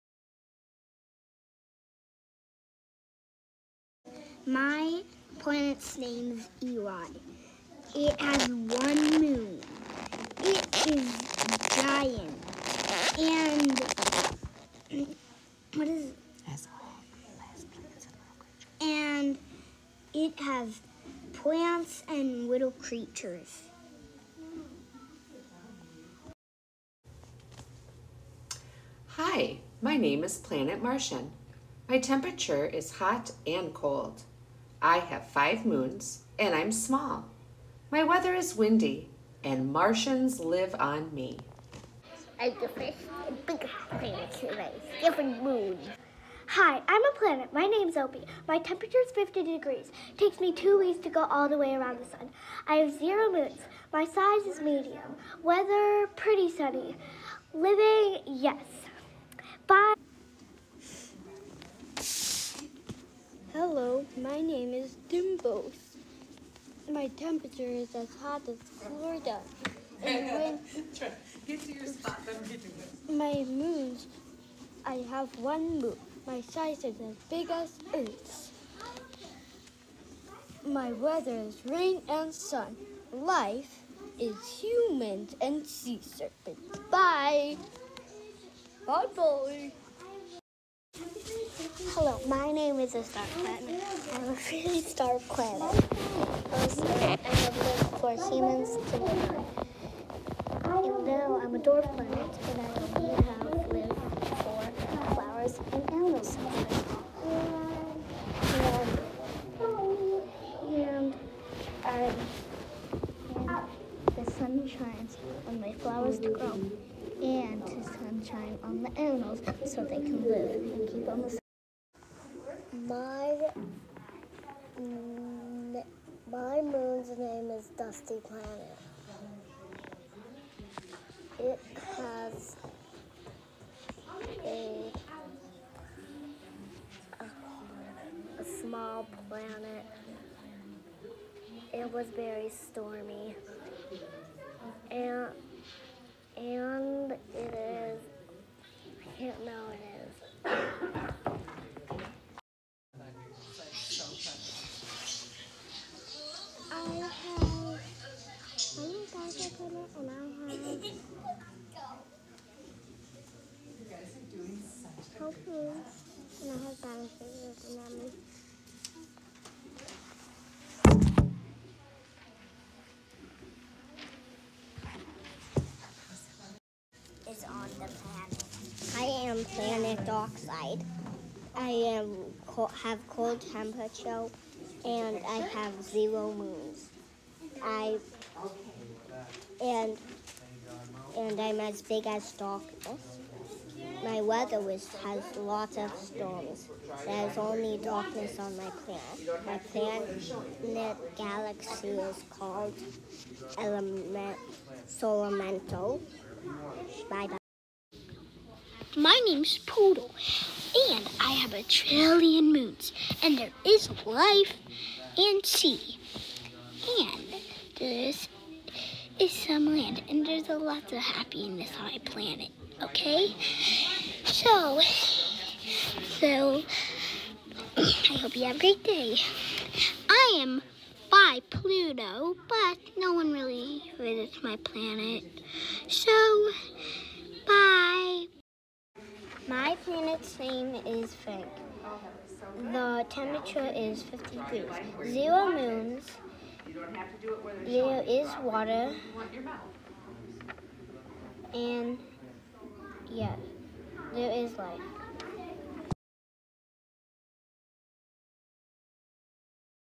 Ms Karen's Class - The Planets 2023
After creating their own planets, Ms. Karen's class used ChatterPix to make their planets come alive.